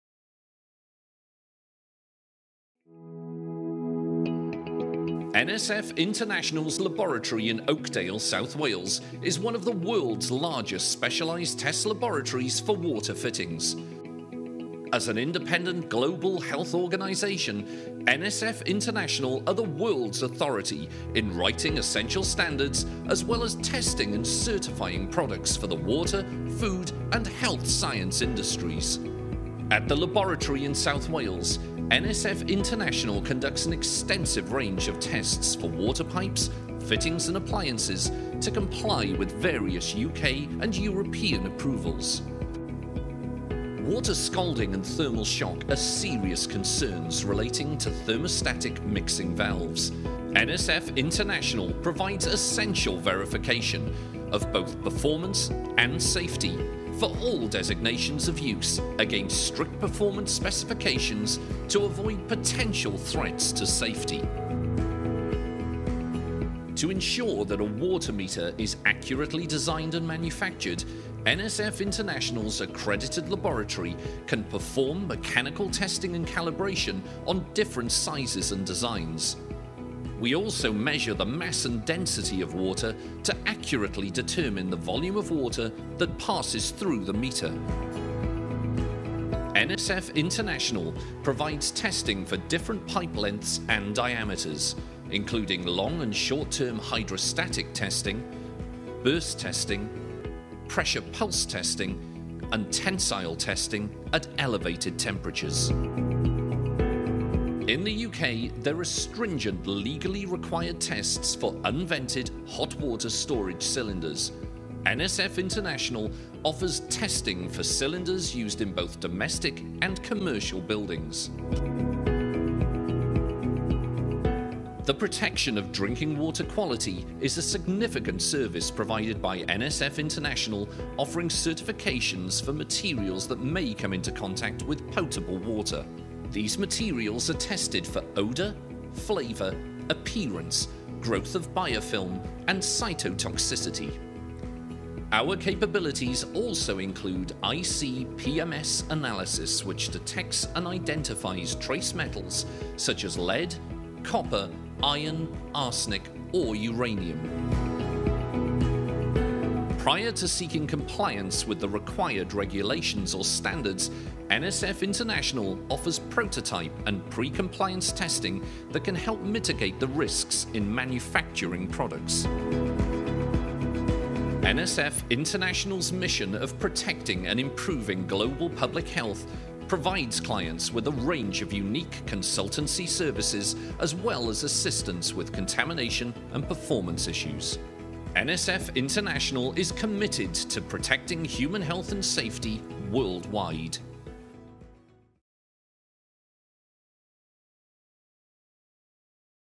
UK Water Laboratory Testing Capabilities | NSF International
NSF International’s laboratory in Oakdale, South Wales is one of the world’s largest specialized test laboratories for water fittings. This laboratory is ISO/IEC 17025 accredited and covers testing for a wide number of products to comply with UK and European approvals.

Products and materials like faucets and faucet accessories, water pipes, thermostatic mixing valves (TMVs), water meters and unvented hot water cylinders, among others, can be tested in this facility.

This laboratory provides services related to UK approvals including NSF REG4, WRAS Product Approvals, BS6920, TMV2 and TMV3. We can also carry out both mechanical and materials tests to a variety of European standards and the Australian WaterMark Certification Scheme.